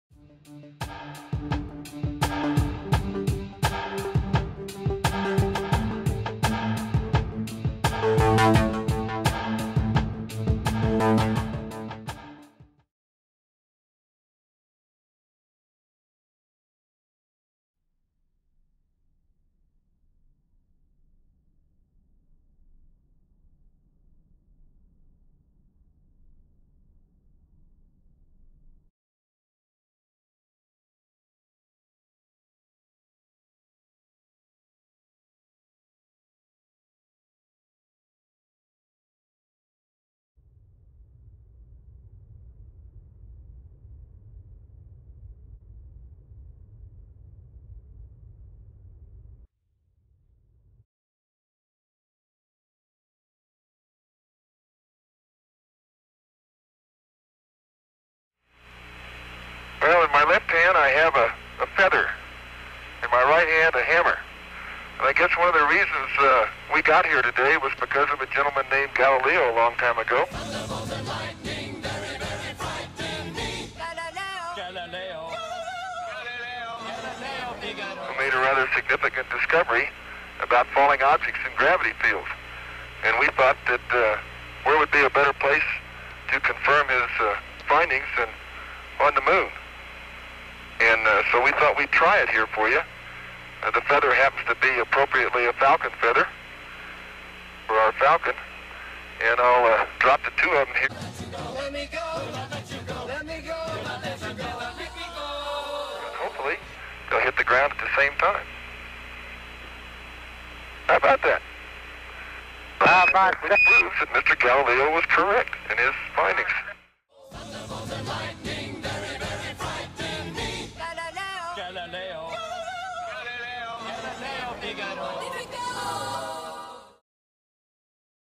Free Fall, Galileo and the Moon
For a long time people thought that heavier things fell quicker when dropped.
Galileo knew different!